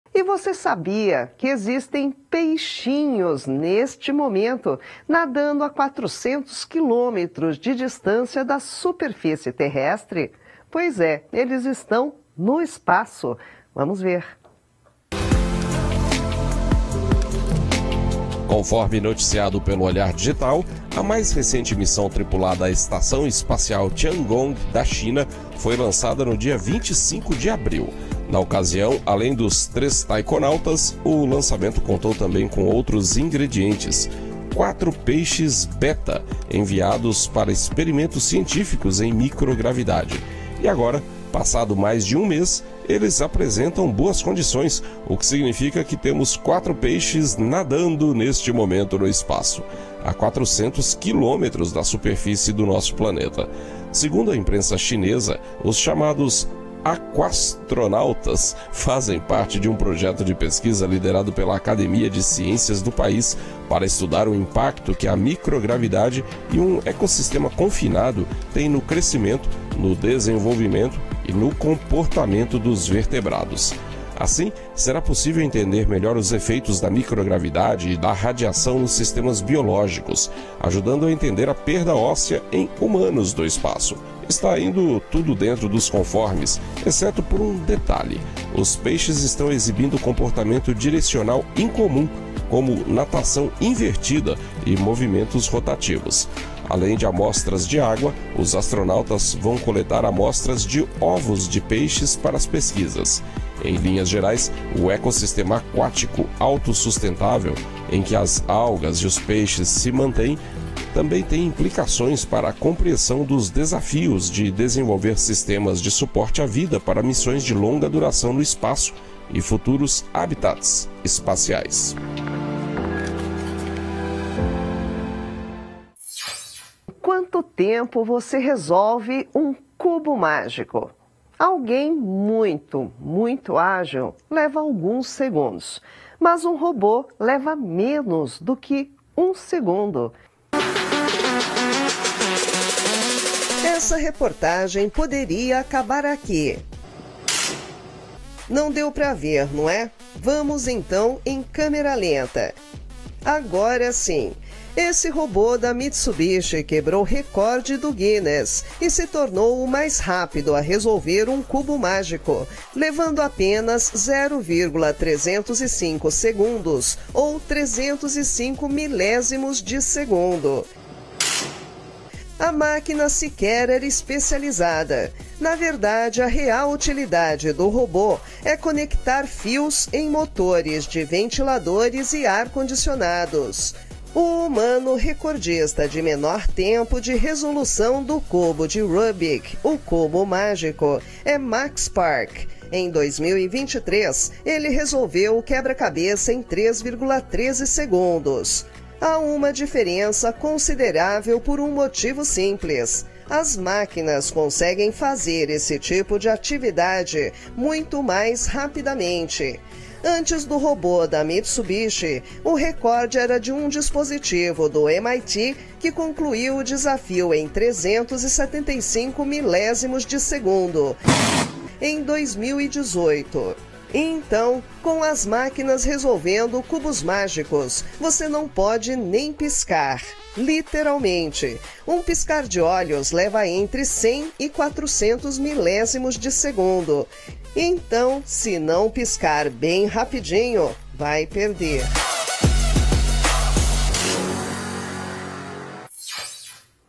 Sabia que há peixes nadando no espaço?
Peixes-zebra exibem comportamento misterioso na estação espacial Tiangong! 😱🐠 O que os astronautas descobriram sobre esses 'aquastronautas'?